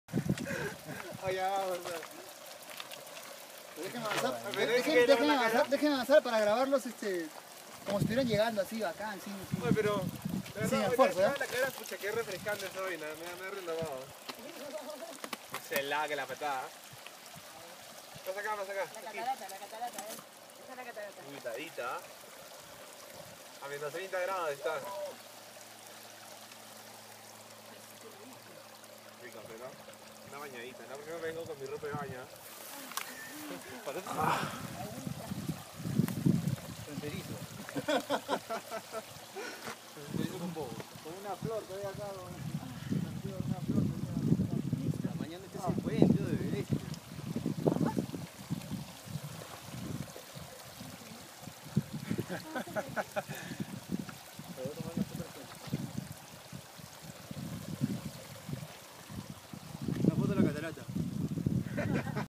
Mini catarata Mortero
aun falta para llegar a la catarata pero no mucho